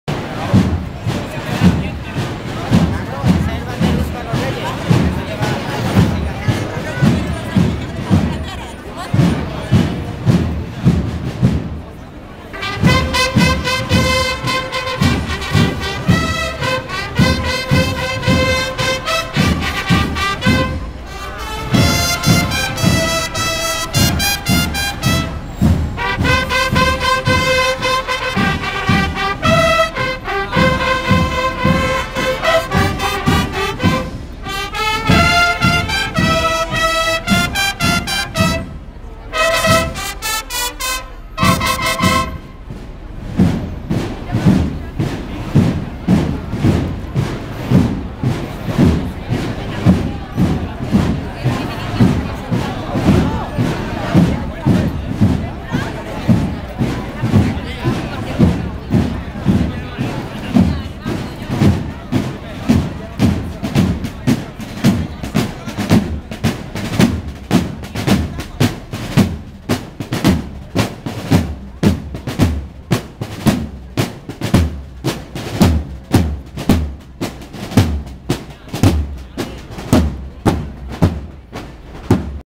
Sevilla - Marching Band
A random marching band making its way through Sevilla's city streets in the pre-Christmas festivities.